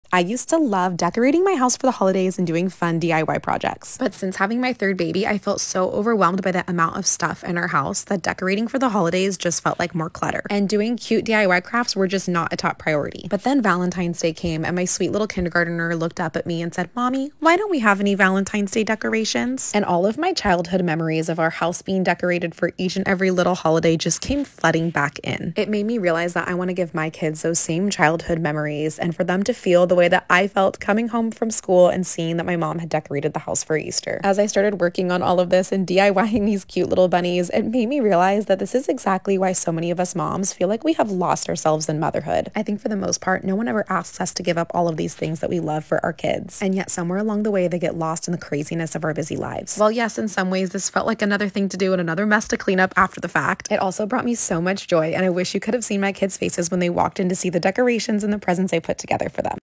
Decorate for Easter With Me!
I’m slowly finding my way back to the things I love that I lost sight of. And I’m doing more of what brings my kids and I joy, one step at a time 🥰

If you’re in a place where you really just can’t bring yourself to do this kind of thing- please know you’re still an amazing mom. I’ve been there 🫶

But if you could use a little more joy and sunshine sometimes you just need to give yourself a kick in the behind, roll up your sleeves and get it done. Often times I over complicate it and put it off when in reality this didn’t take long and it was so worth it for the happiness it brought us all!

P.S. don’t forget to teach your kids the REAL reason we celebrate Easter! The books I gifted my kids are all Christian Easter books and I highly suggest them for every family with young kids!

You are not required to use them and using them doesn't affect you as a customer. It just helps me continue to make and share content with you!